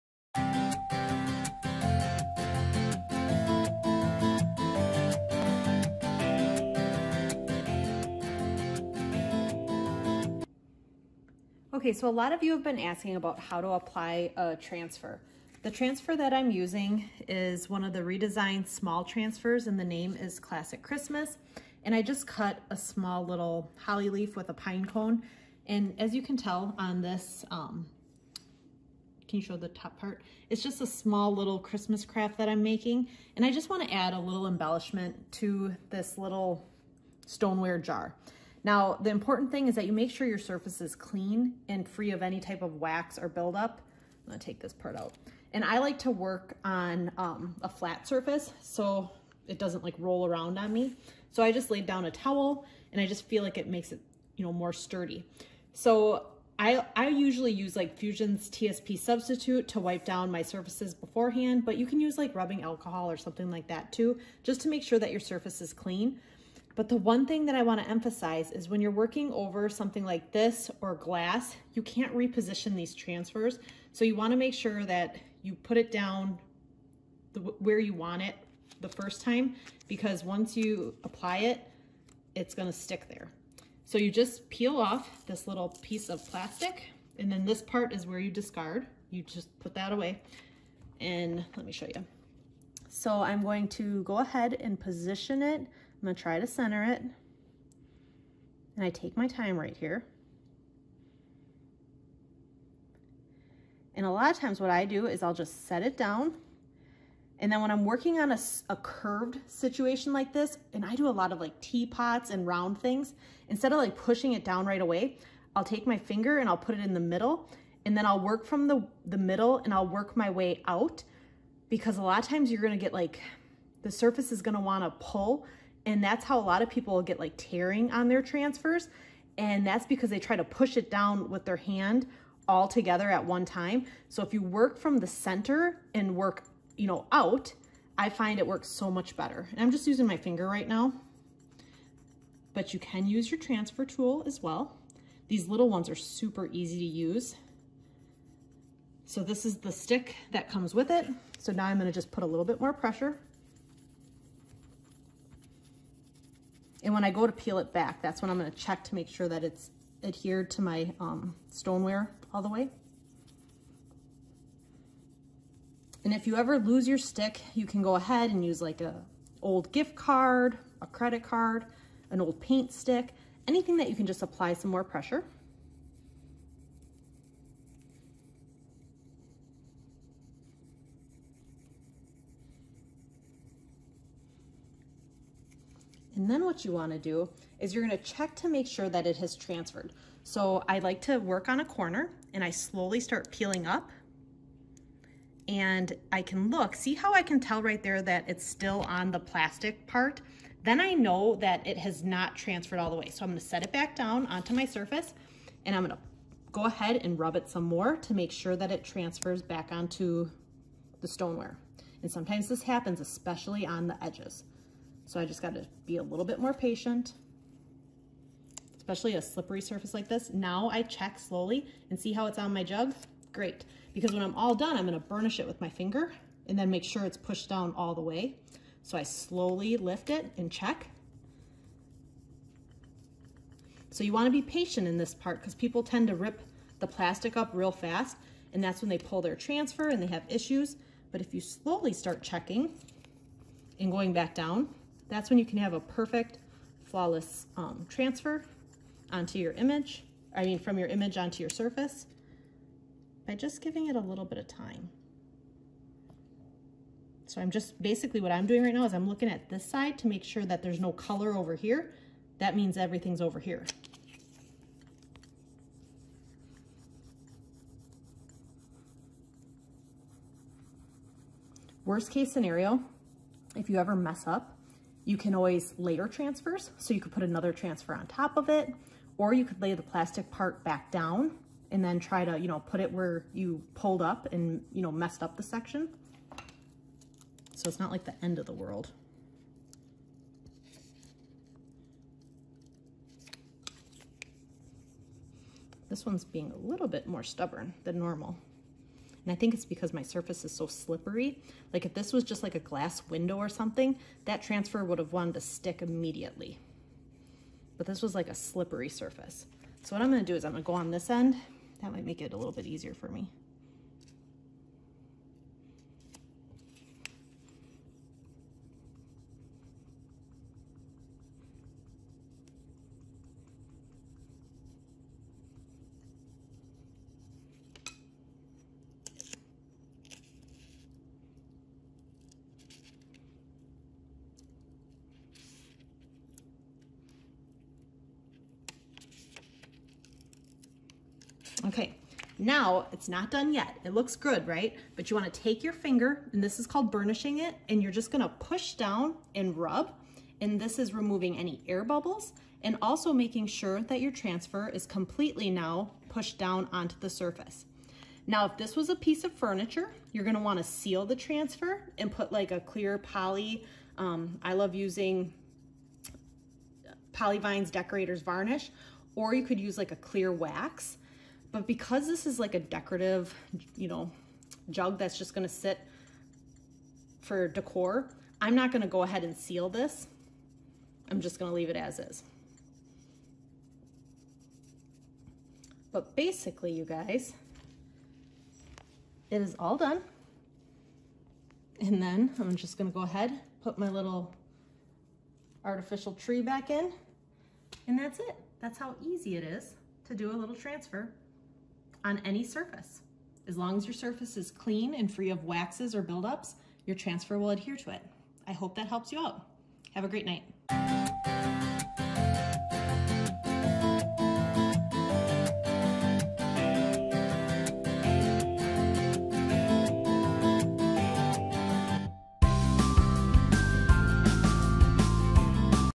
How To Apply A Redesign With Prima Transfer
Learn how to quickly apply a Redesign with Prima Transfer.  You can add transfers to your home décor and furniture using this simple method.
For all other products including Fusion Mineral Paint, Milk Paint, Redesign with Prima, Polyvine and More